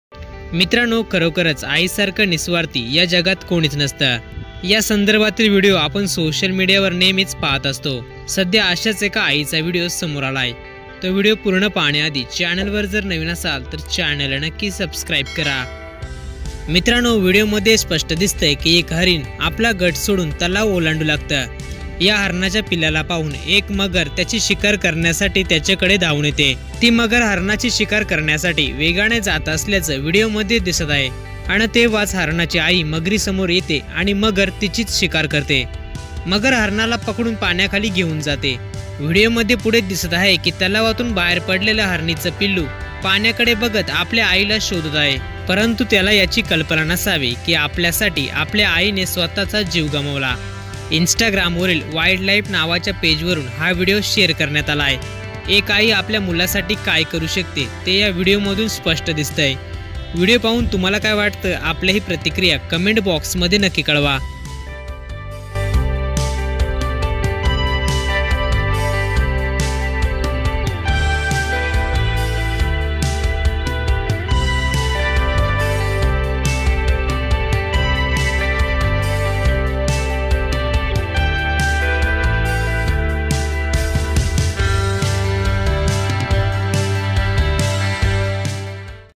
शेवटी आईच ना ती; आपल्या पिल्लाला वाचवण्यासाठी हरीण स्वतः मगरीशी लढली, हृदयस्पर्शी व्हिडिओ.
marathinews
maharashtranews
thanelivenews
maharashtralivenews
todaylivenewsmarathi
kalyannews
youtubemarathinews
todaynewsepisode
tv9marathinews
zee24taasnews
abpmazanews
allmarathinews